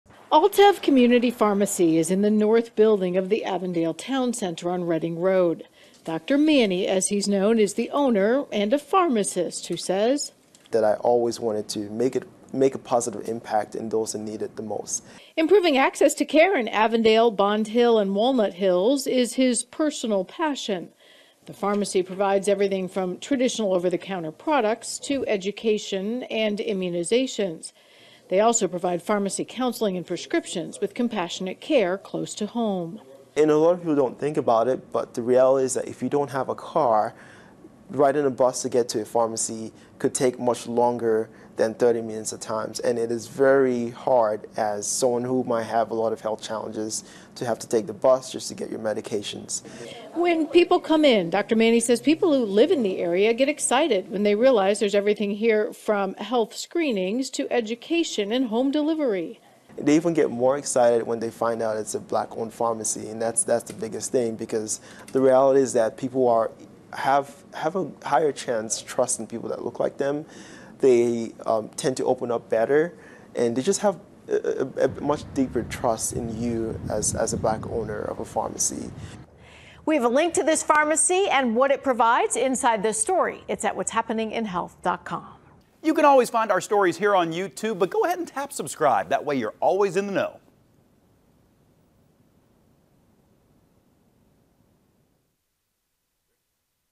Black-owned community pharmacy now operating in Cincinnati
A new pharmacy has a mission of care that goes beyond prescriptions